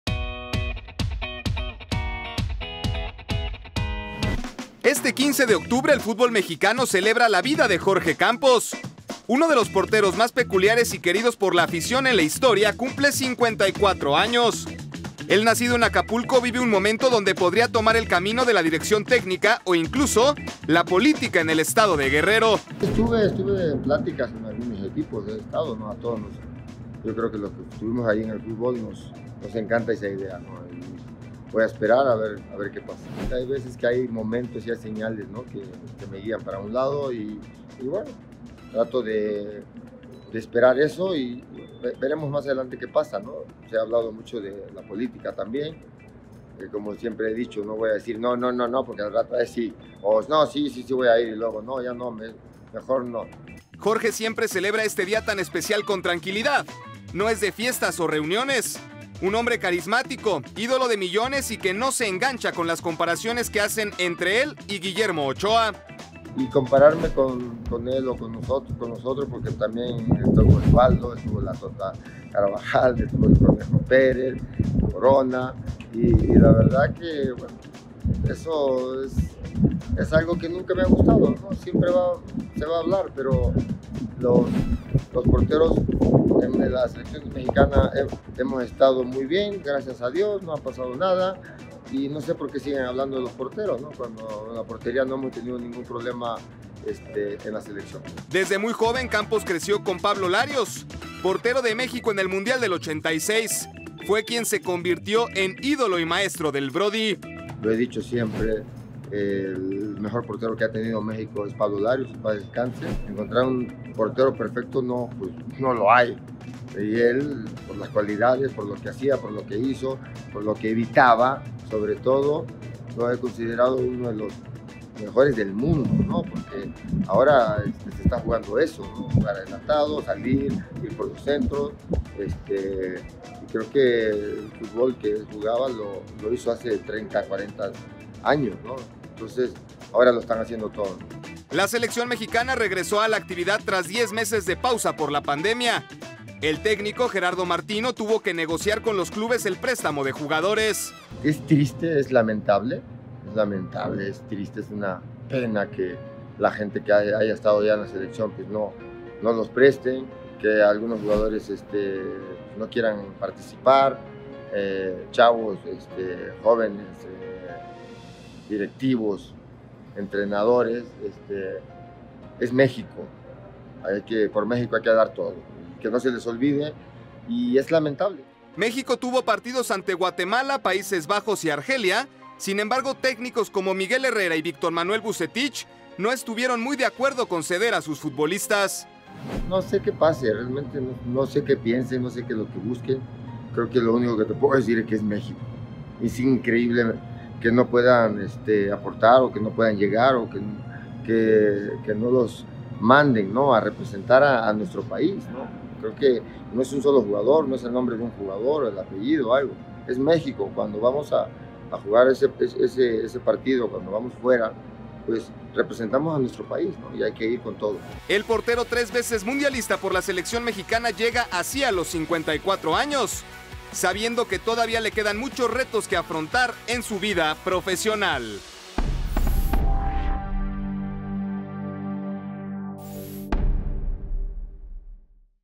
Jorge Campos: “¿Memo Ochoa o yo?… El mejor fue Pablo Larios”
El ‘Brody’ que hoy cumple 54 años confiesa a Latinus que ha tenido varias ofertas para dirigir, e incluso lo han tentado para que incursione en la política.